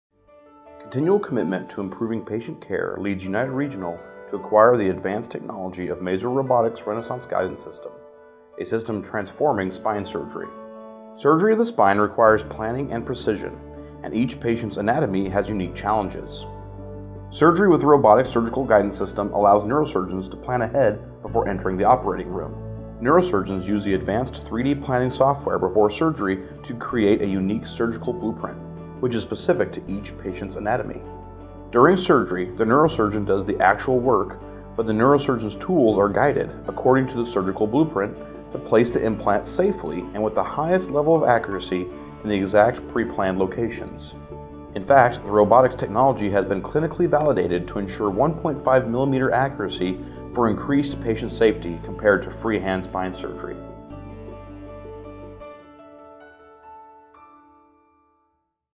Spine Surgery - The Mazor Robotics Renaissance Guidance System
Surgery of the spine requires planning and precision, and each patient’s anatomy has unique challenges. Surgery with the robotic surgical guidance system allows neurosurgeons to plan ahead before entering the operating room.

Neurosurgeons use the advanced 3D planning software before surgery to create a unique surgical blueprint, which is the ideal surgery for each patient’s condition. During surgery, the neurosurgeon does the actual work but the neurosurgeon’s tools are guided, according to the surgical blueprint, to place the implants safely and with the highest level of accuracy in the exact preplanned locations. In fact, the robotics technology has been clinically validated to ensure 1.5mm accuracy for increased patient safety compared to freehand spine surgery. And since there is no need for additional X-rays during surgery, with Renaissance™ there is less exposure to radiation than with other surgical treatments.

This robotic surgical guidance system is not independent and does not cut or perform any action on the body. Only the neurosurgeon performs the required procedures such as placing implants. The neurosurgeon is in full control of the system at all times.